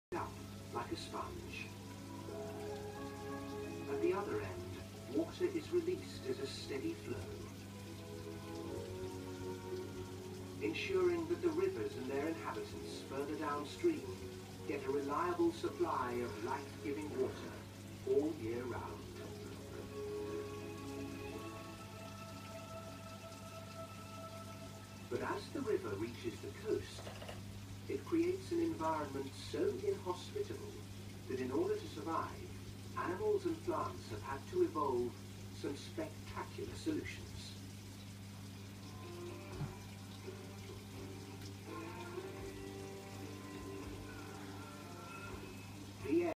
Clown fish hosting ricordia mushrooms.
Two clownfish hosting a group of green ricordia mushrooms in reef tank.